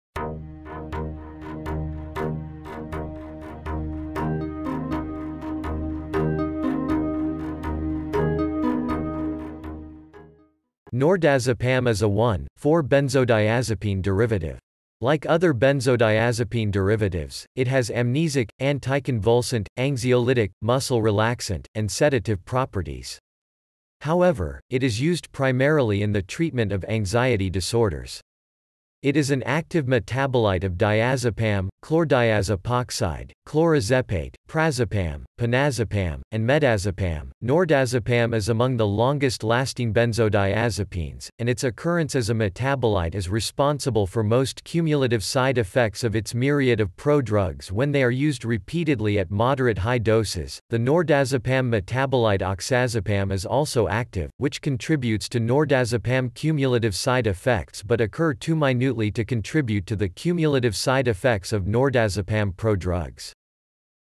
What is Nordazepam? Explain Nordazepam, Define Nordazepam, Meaning of Nordazepam
~~~ Nordazepam ~~~

Title: What is Nordazepam? Explain Nordazepam, Define Nordazepam, Meaning of Nordazepam

Description: Nordazepam is a 1,4-benzodiazepine derivative. Like other benzodiazepine derivatives, it has amnesic, anticonvulsant, anxiolytic, muscle relaxant, and sedative properties. However, it is used primarily in the treatment of anxiety disorders. It is an active metabolite of diazepam, chlordiazepoxide, clorazepate, prazepam, pinazepam, and medazepam.Nordazepam is among the longest lasting benzodiazepines, and its occurrence as a metabolite is responsible for most cumulative side-effects of its myriad of pro-drugs when they are used repeatedly at moderate-high doses; the nordazepam metabolite oxazepam is also active , which contributes to nordazepam cumulative side-effects but occur too minutely to contribute to the cumulative side-effects of nordazepam pro-drugs .